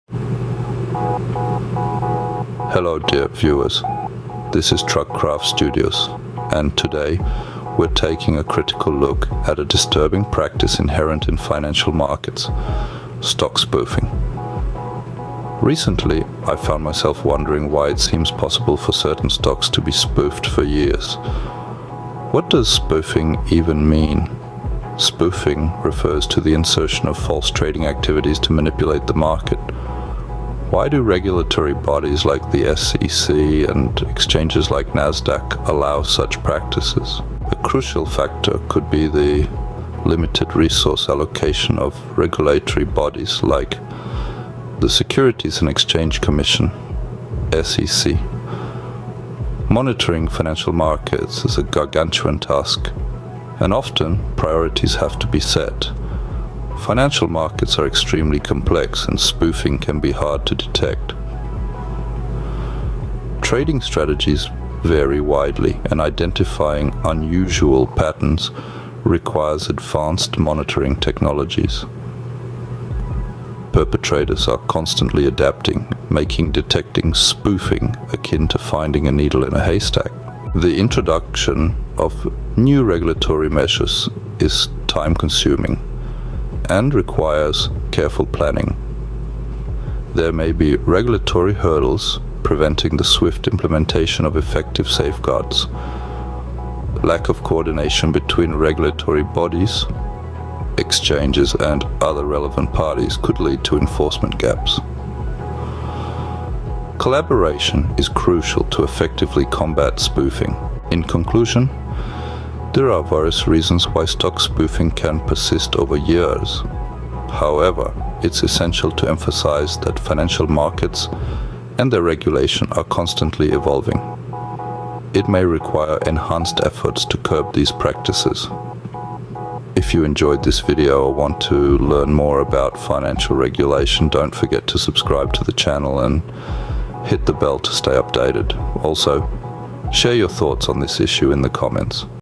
The Dark Side of Financial Markets: Unmasking Stock Spoofing
Description:

Welcome to TruckCraftStudios! In today's investigative journey, we dive deep into the murky waters of financial markets. 🕵️‍♂️ Join me, Chris, as we explore the unsettling world of stock spoofing and question why it seems to persist for years.

📈 Why are certain stocks allowed to be spoofed over extended periods? In this eye-opening video, we dissect the complexities of financial markets, regulatory challenges faced by entities like the SEC, and the intricacies of combating spoofing practices.

🔍 Key Points Covered:

Limited Oversight Resources: Understand the challenges faced by regulatory bodies like the SEC due to the mammoth task of monitoring vast financial landscapes with limited resources.

Complexity of Financial Markets: Delve into the intricacies of the financial markets, where advanced spoofing techniques can be elusive, making detection akin to finding a needle in a haystack.

Regulatory Hurdles: Explore the time-consuming process of implementing new regulatory measures and the potential hurdles that hinder swift action against spoofing.

Collaboration Gaps: Uncover the importance of collaboration between regulatory bodies, exchanges, and other stakeholders to effectively curb and prevent stock spoofing.

🌐 Stay Informed: The world of finance is ever-evolving, and understanding the dynamics is crucial.

What are your views on stock spoofing, and do you think enough is being done to address it?

Thanks for being part of our investigative journey! 🚀